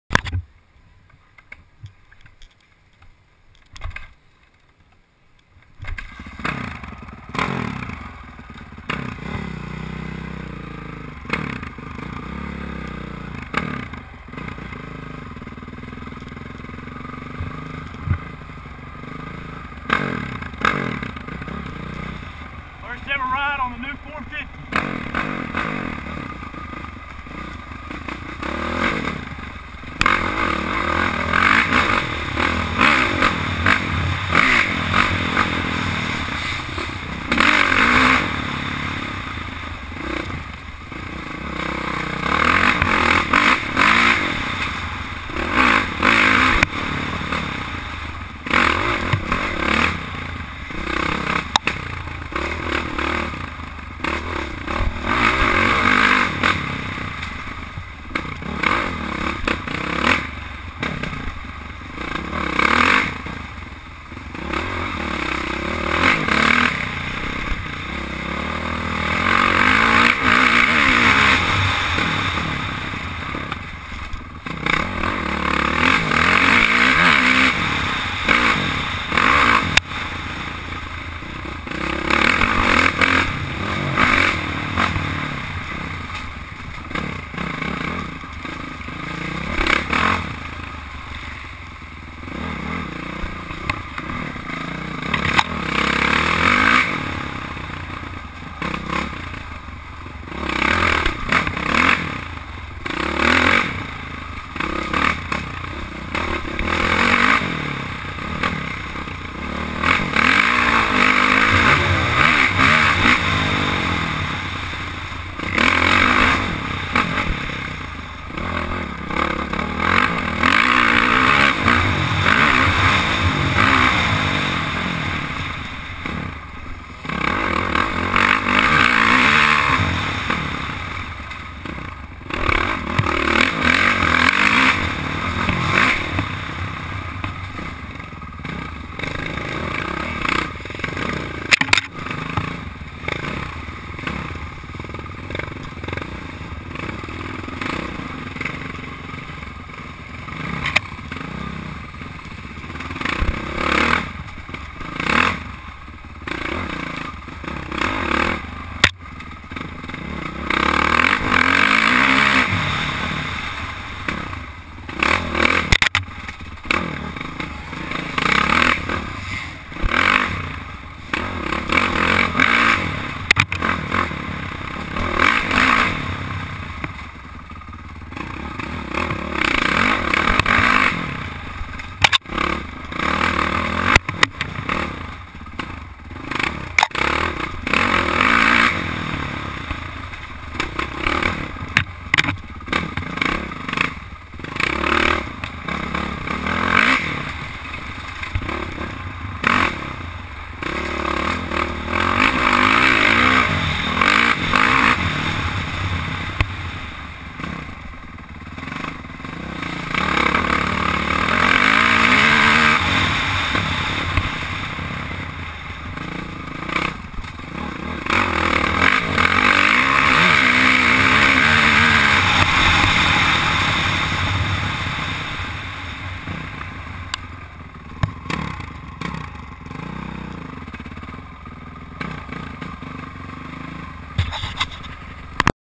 Crf 450 first ride
ridin my new used bike at badlands